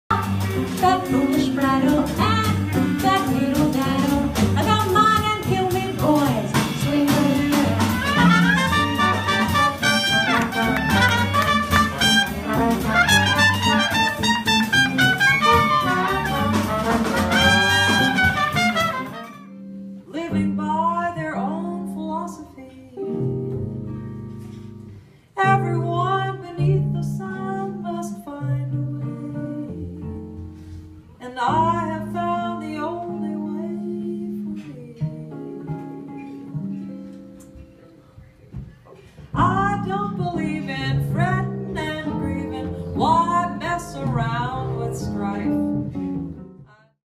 Petra's Recession Seven and Alexis Cole at Winter's Jazz Club
Petra's Recession Seven and Alexis Cole at Winter's Jazz Club this weekend!